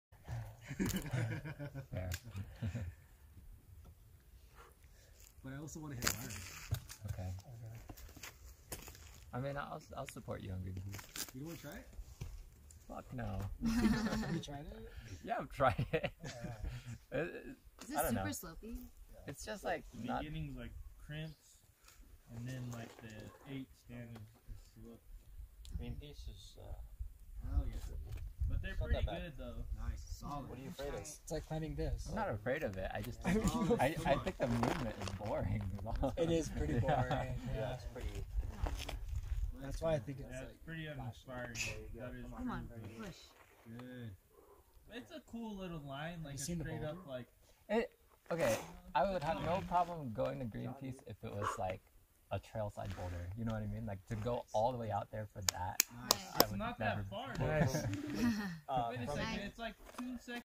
Tramway Bouldering | Slitherin (V5)
Bouldering with friends in Tramway